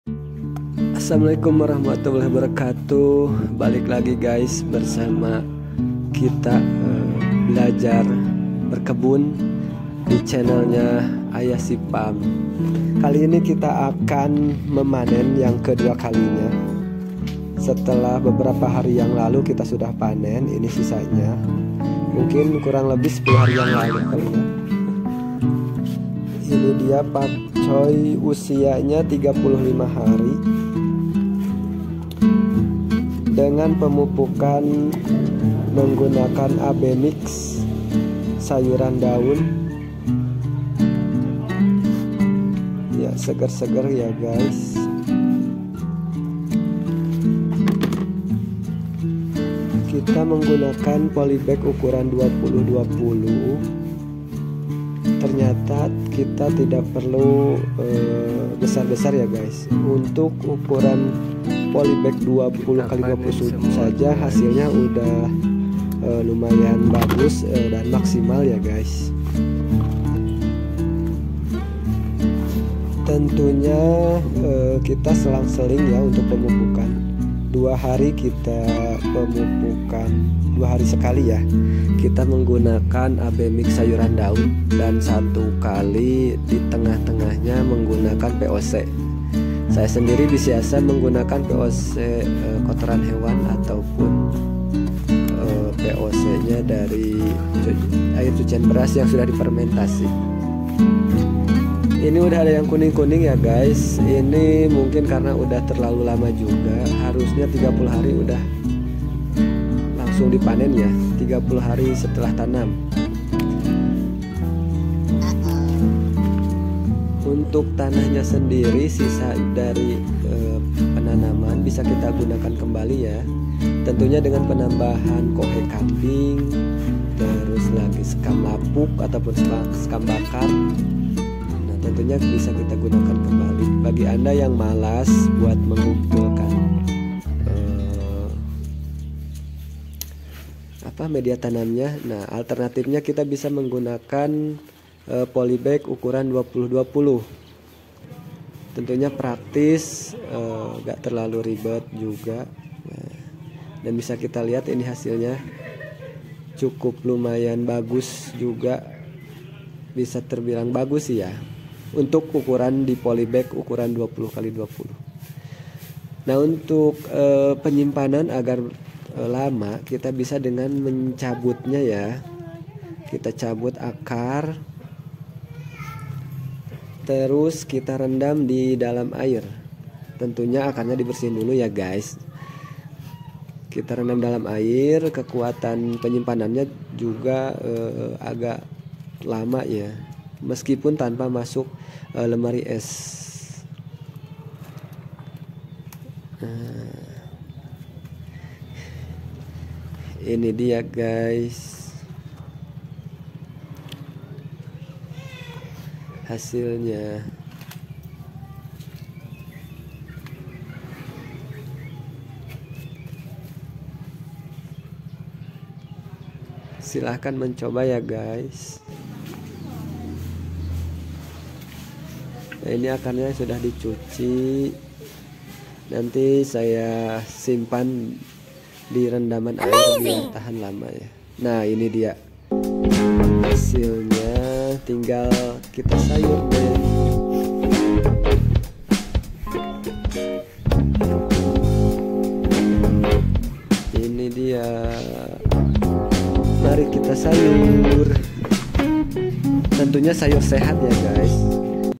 cara panen sawi pakcoy di polybag bisa panen berkali-kali
Panen sawi pakcoy di polybag bisa panen berkali-kali
bisa awet dan hemat gak perlu ke pasar
 setelah sblumnya kita panen tipis2 nah sekarang kita paneh habiskan guys.. sayur pakcoy nya seger2 bergizi lagi..

Gak perlu kawatir habis, nanti kita tanam lagi guys, semangat berkebun guys, jangan sampai halaman rumahmu  terbengkalai ya guys. mari kita hijaukan halaman rumahmu dengan berkebun menanan sayuran...
ayyashifarm.